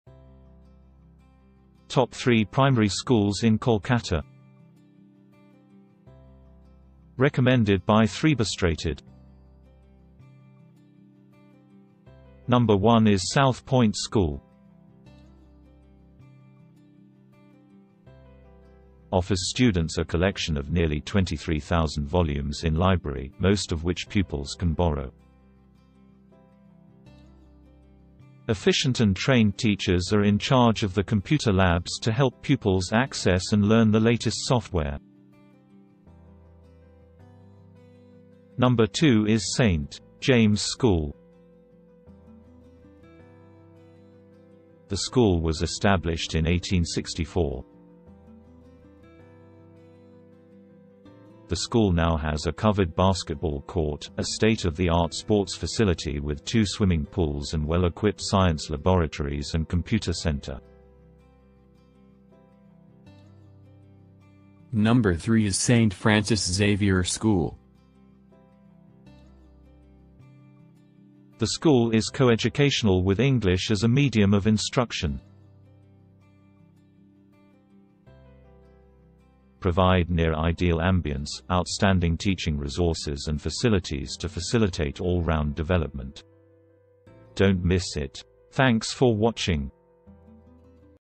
Top 3 Primary Schools in Kolkata
South Point School
St. James' School
St. Francis Xavier School

The Best Primary Schools in Kolkata by Threebestrated based on proprietary 50-Point Inspection includes everything from checking customer reviews, history, complaints, ratings, satisfaction, trust and cost to the general excellence.